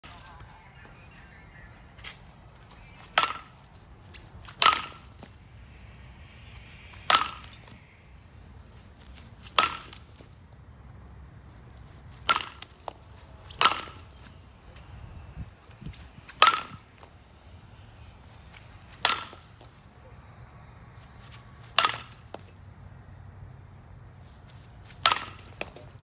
2022, Uncommitted Speedy Slapper/ Versatile Outfielder, JaKeyla McDaniel
Getting some backyard tee work in. Working on getting better after a tough loss and outing